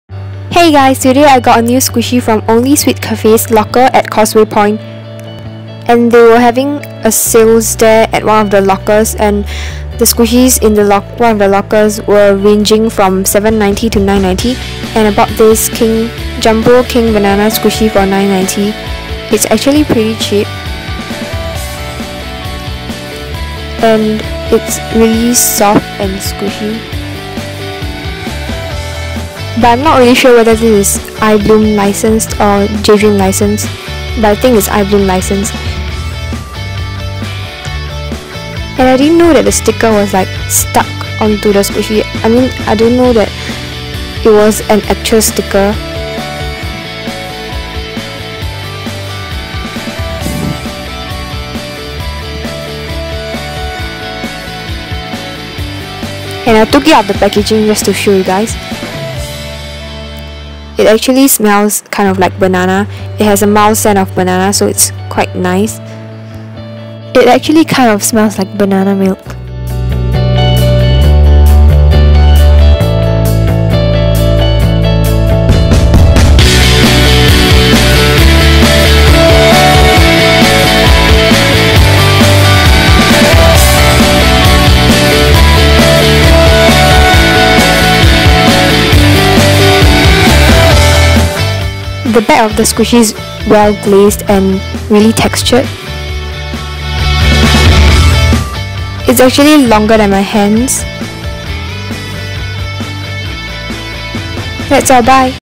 Squishy Haul #3 🍌(HB)
Hey guys!:) so I got tis new banana squishy from onlysweetcafe's locker from cwp and hope u like tis short vid ! -HB